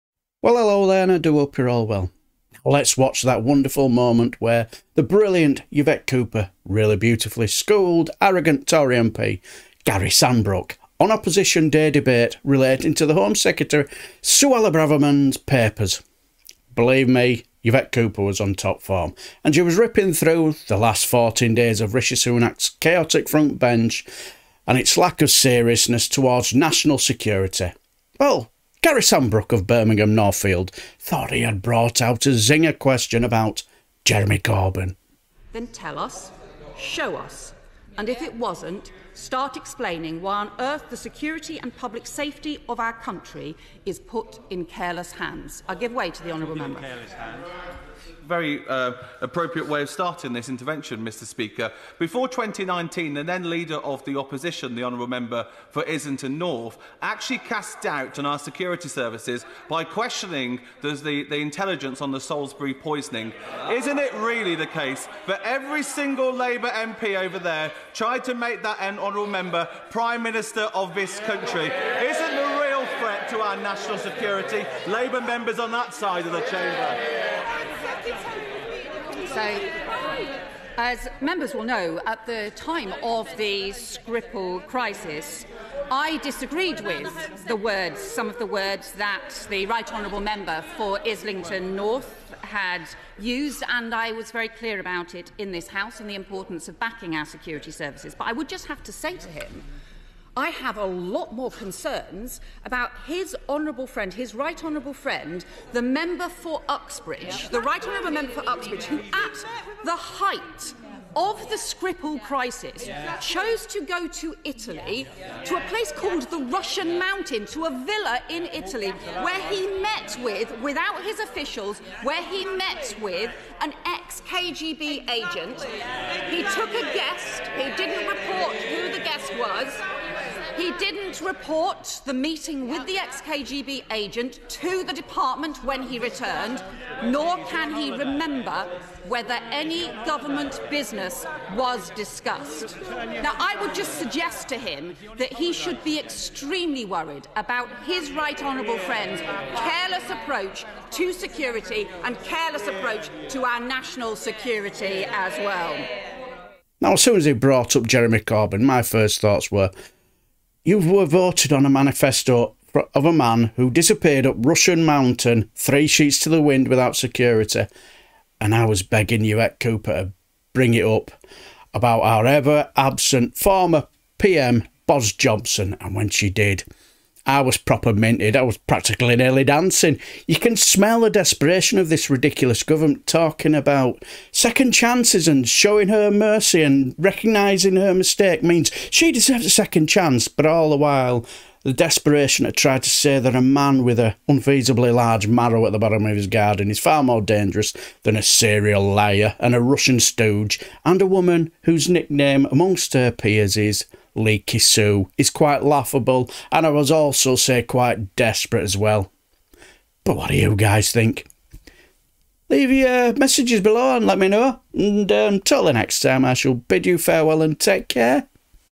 Brilliant Yvette Cooper Really Beautifully Schooled Arrogant Tory MP?
0:00 Introduction
0:35 Brilliant Yvette Cooper Really Beautifully Schooled Arrogant Tory MP?
2:37 Final Thoughts
Now let's watch that wonderful moment where the brilliant Yvette Cooper, really beautifully schooled Arrogant Tory MP Gary Sambrook, on opposition day debate, relating to the Home Secretary Suella Braverman Papers? Believe me Yvette Cooper was on top form.  As she was ripping through the last 14 days of Rishi Sunak's chaotic front bench and it's lack of seriousness towards national security, Gary Sambrook of Birmingham Northfield, thought he had bought out a zinger question about, Jeremy Corbyn?